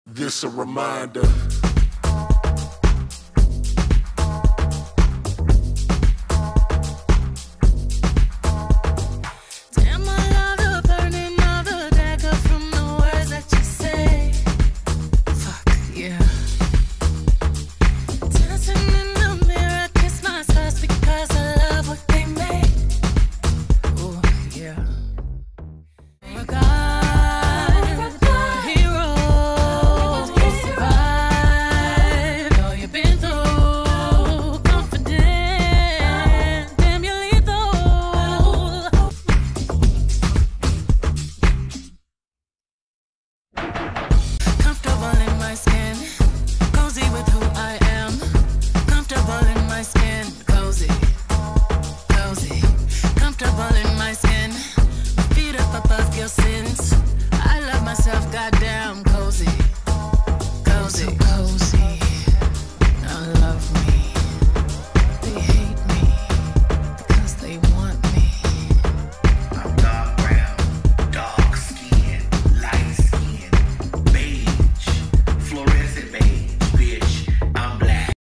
I'M COZY WITHIN MY SKIN😁!
BE COMFORTABLE WITH WHO YOU POSITIVELY ARE, BUT MOST IMPORTANTLY, BE COZY WITHIN THE SKIN, GOD CREATED YOU IN, because;

Because I'M DARK BROWN, DARK SKIN, LIGHT SKIN, BEIGE, FLUORESCENT BEIGE! BITCH, I'M BLACK✊🏿!
MY CHOCOLATE MELANIN SKIN😁!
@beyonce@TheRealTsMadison